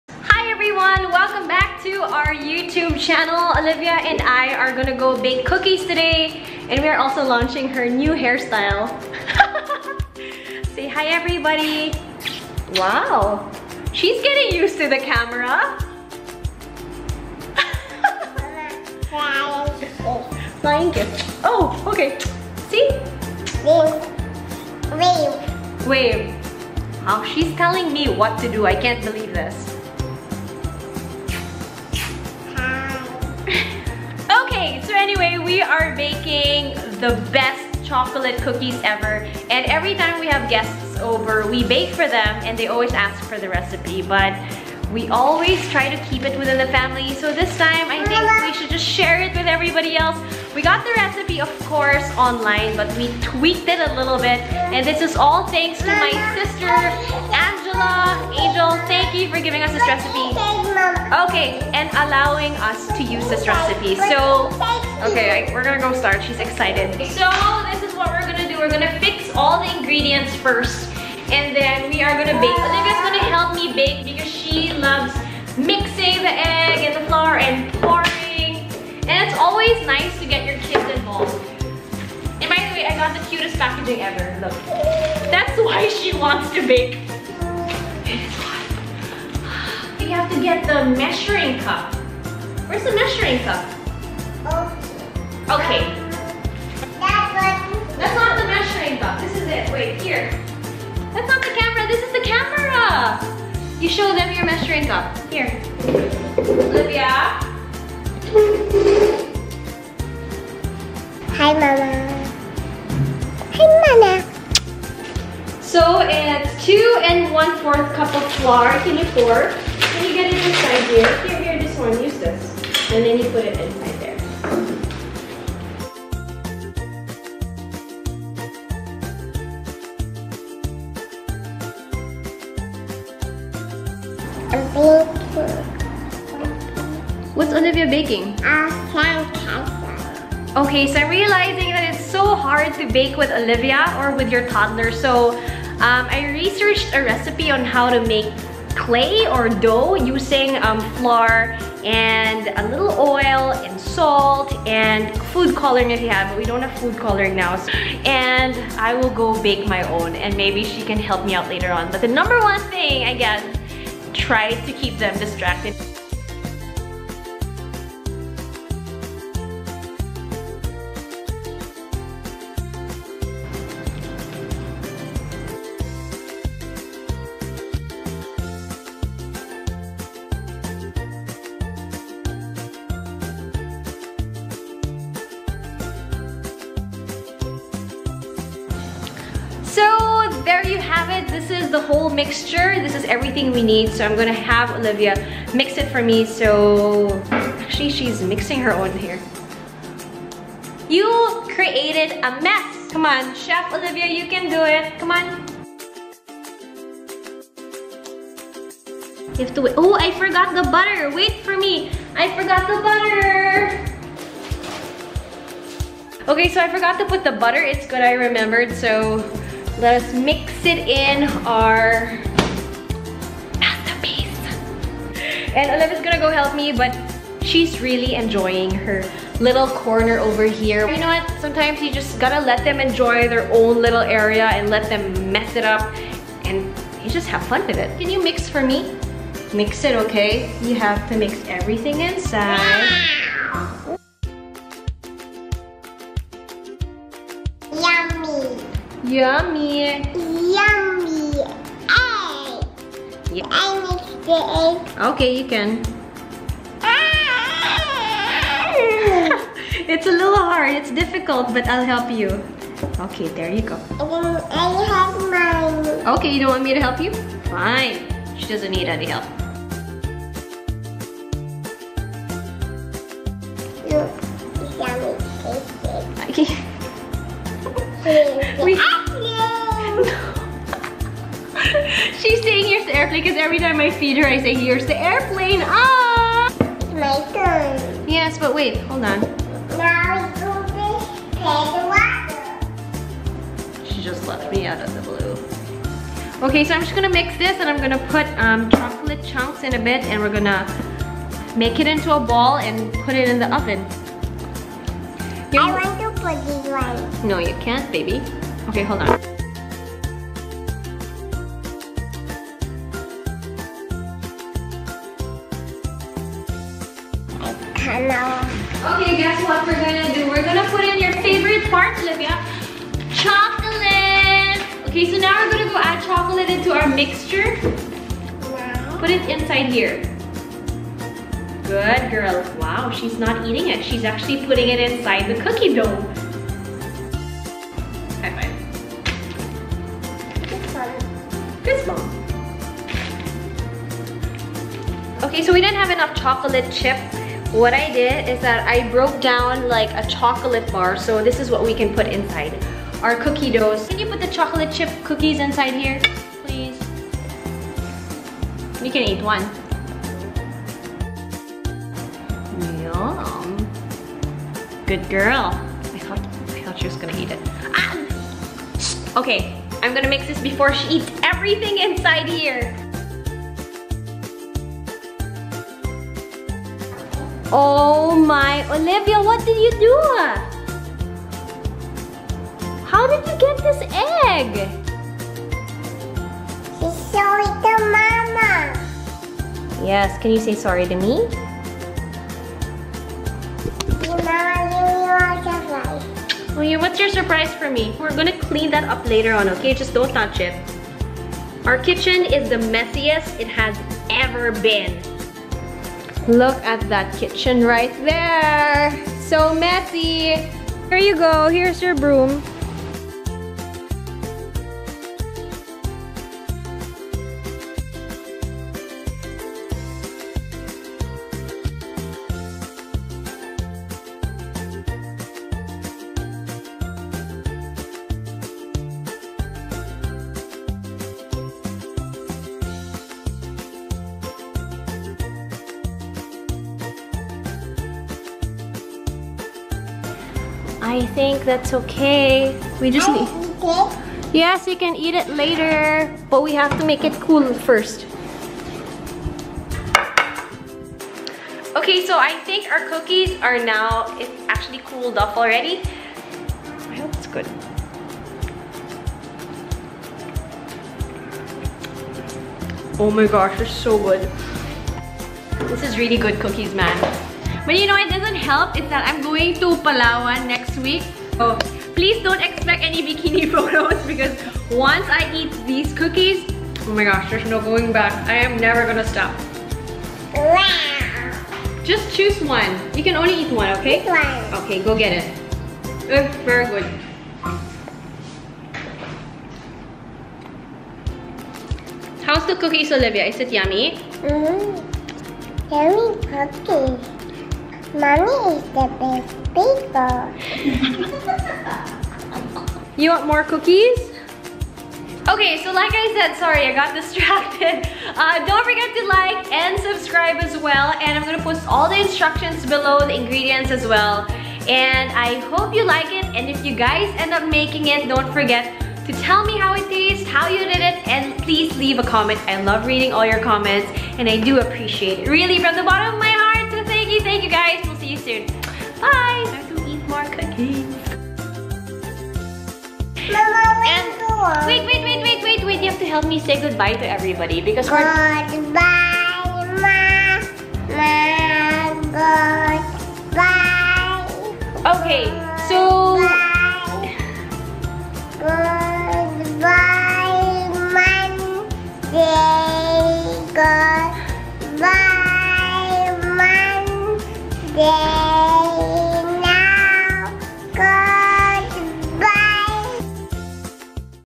The Best Chocolate Chip Cookies + Baking with your Toddler | Andi Manzano
2 and 1/4 cups all-purpose flour
1 teaspoon baking soda
1 and 1/2 teaspoons cornstarch
1/2 teaspoon salt
3/4 cup unsalted butter, melted1
3/4 cup loosely packed light brown sugar
1/2 cup white sugar
1 large egg + 1 egg yolk
2 teaspoons pure vanilla extract
1 cup chocolate chips or chocolate chunks

Preheat oven to 325°F (163°C). Bake for about 8-10 minutes. (8 if you want it chewy)!

Let me know if you were able to make it!